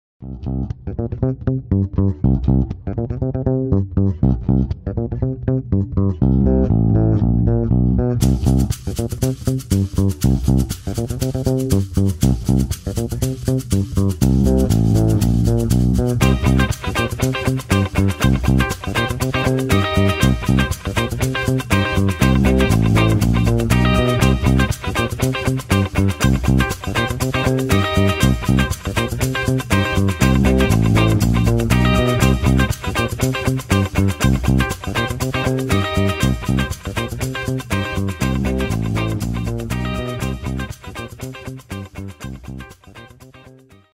Delete Empty Cells - VBA Macros - Tutorial - MS Excel 2007, 2010, 2013
This macro will delete all empty cells.